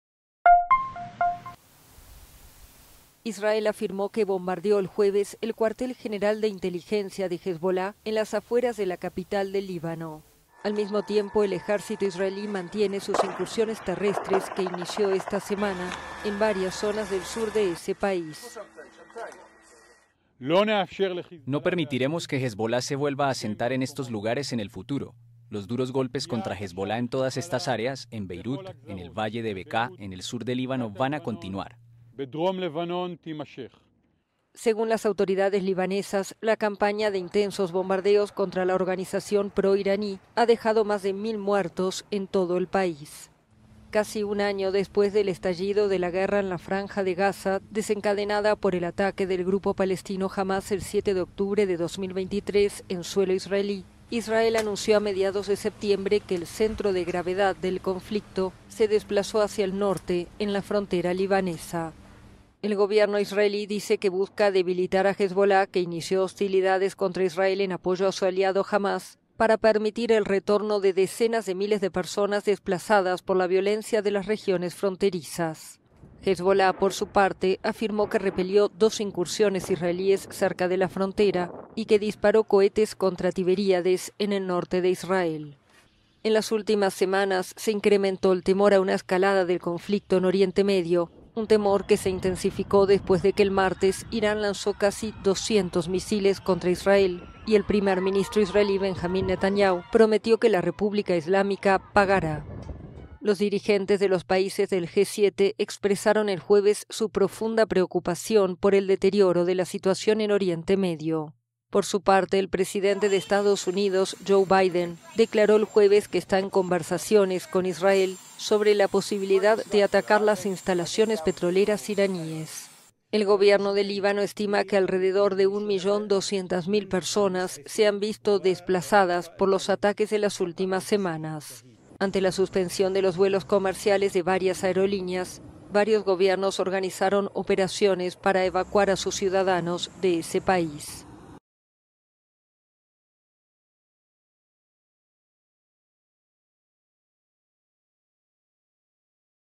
Israel bombardeó cuartel general de inteligencia de Hezbolá
Israel afirmó que bombardeó el cuartel general de inteligencia de Hezbolá en Beirut.